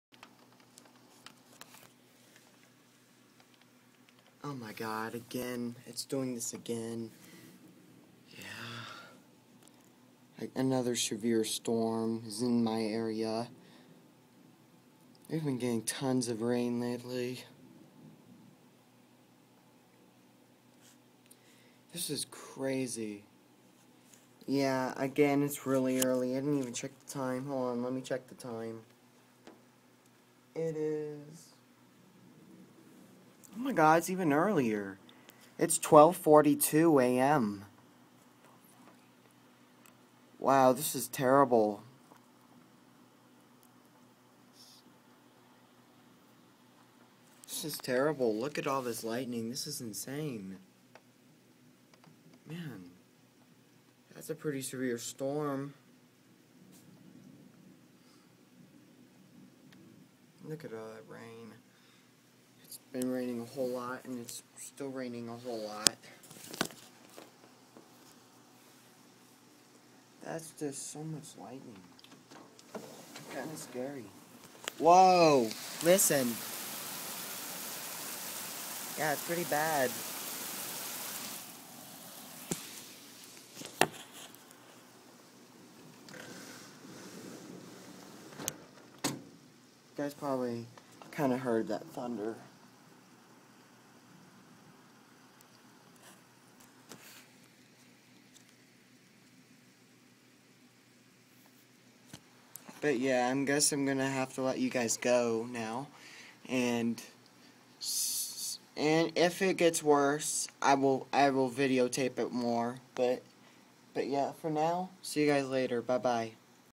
Another stormy morning 8/26/14
This is actually worse than last night we got more rain and tons more lightning